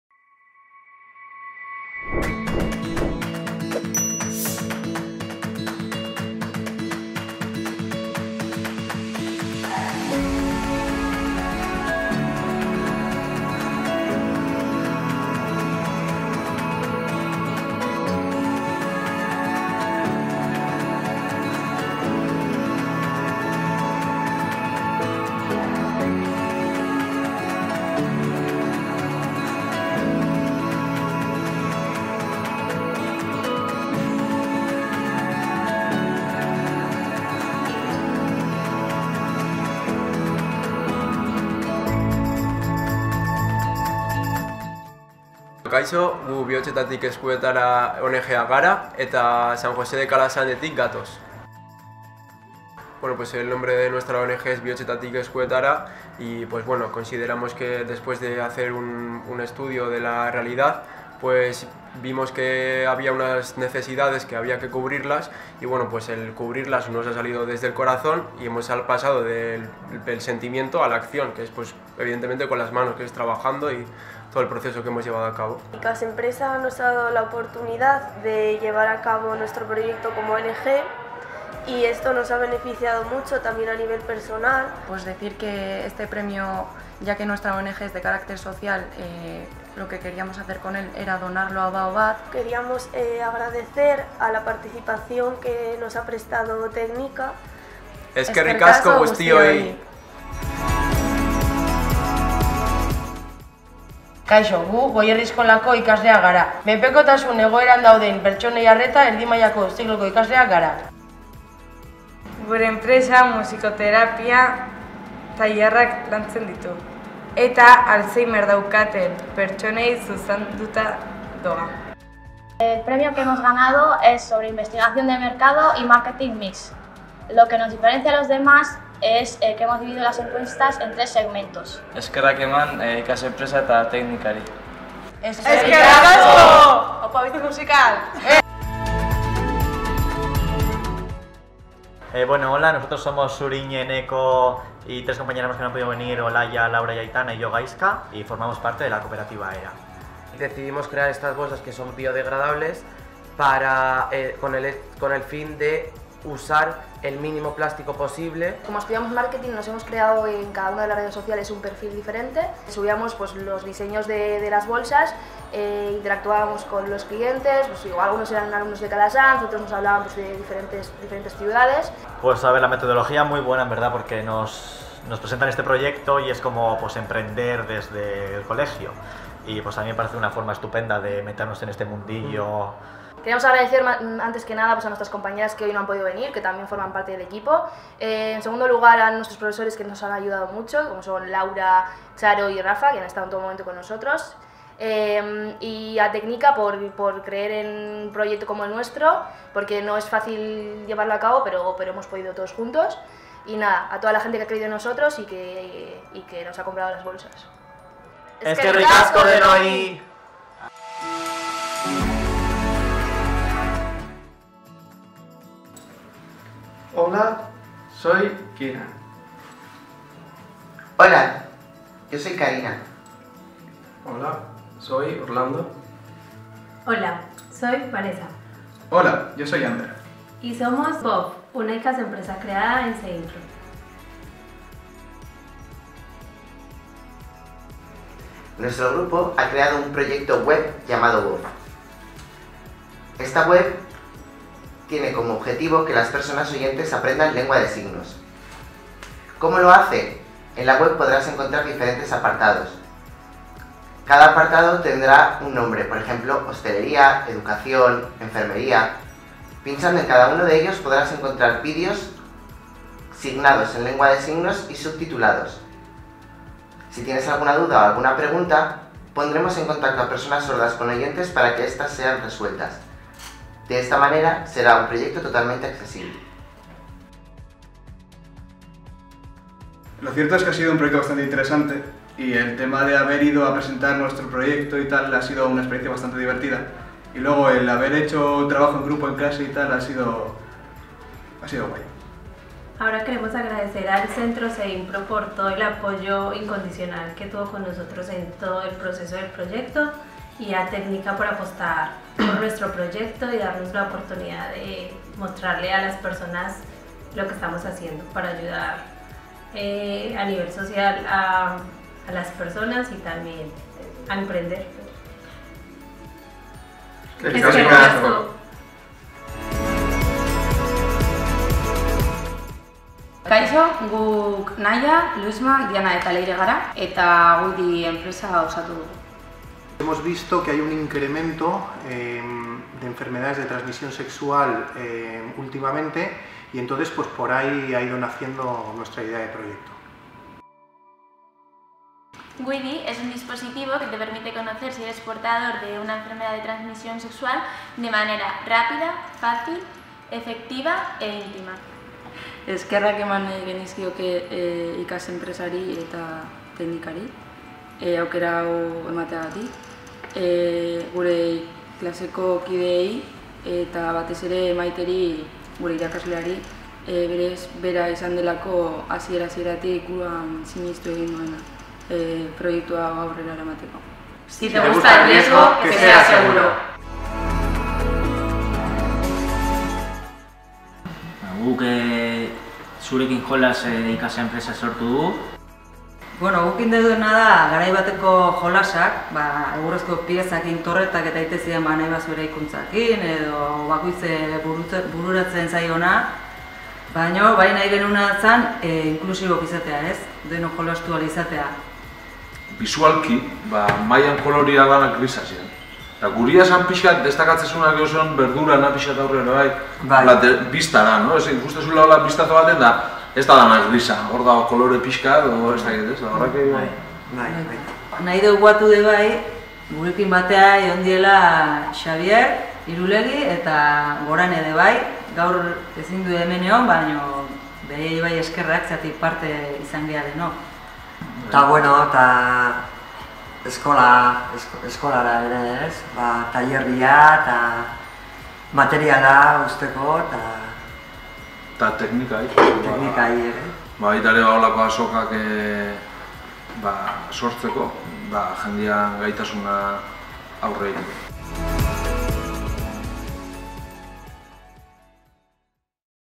ekintzailetzasariak 2019 Ikasenpresa Irabazleak - Ganadores
WIDI -CIFP Tartanga LHII:  Idea de negocio / Negozio ideia

Bizi Musical - CPES Goierri BHIP : Estudio de mercado y Marketing Mix / Merkatu ikerketa eta  Marketing Mix-a

Cooperativa Hera - San Jose de Calasanz: Impacto social campaña de lanzamiento y divulgación / Inpaktu soziala, merkaturatze eta zabalkunde kanpaina

Zurekin Jolas  - Izarraitz Lanbide Heziketa :Punto de Venta y merchaindising / Salmenta-puntua eta merchandising-a

VOFF   - Ceinpro : Idea de negocio con proyección de futuro / Etorkizun handiko negozio ideia

Bihotzetatik Eskuetara -  San José de Calasanz: Emprendimiento social / Ekintzailetz soziala